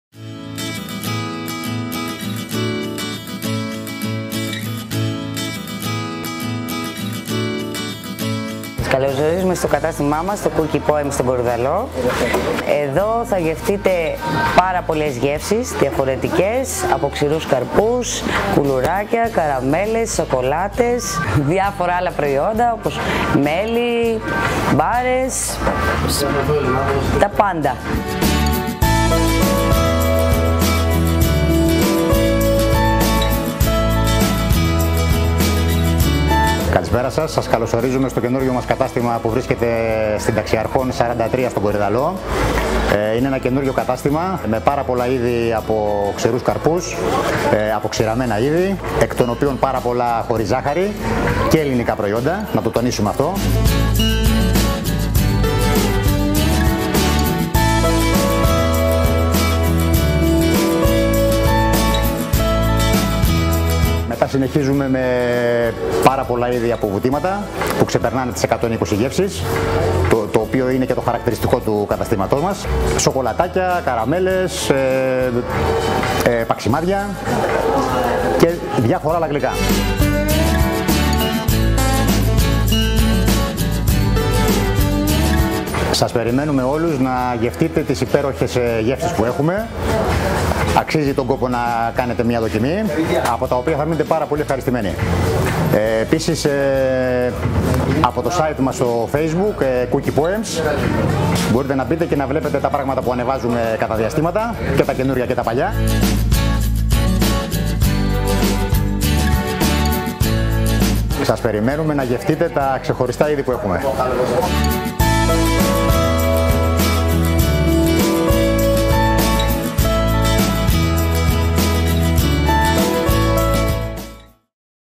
Cookie Poems στον Κορυδαλλό
Κουλούρια, καραμέλες, σοκολάτες, αποξηραμένα φρούτα, παγωτό, κρασί, προϊόντα χωρίς γλουτένη και πολλά άλλα...
Το Cookie Poems στον Κορυδαλλό είναι ένα νέο κατάστημα! Σας προσκαλεί να δοκιμάσετε τις μοναδικές του γεύσεις και σας υπόσχεται την απόλαυση σε όλο της το μεγαλέιο!
Εάν αγαπάς τις λαχταριστές γεύσεις,δεν έχεις παρά να επισκεφθείς το Cookie Poems στον Κορυδαλλό!

Cookie Poems
Ταξιαρχών 43 Κορυδαλλός
ΤΚ 18120, Αθήνα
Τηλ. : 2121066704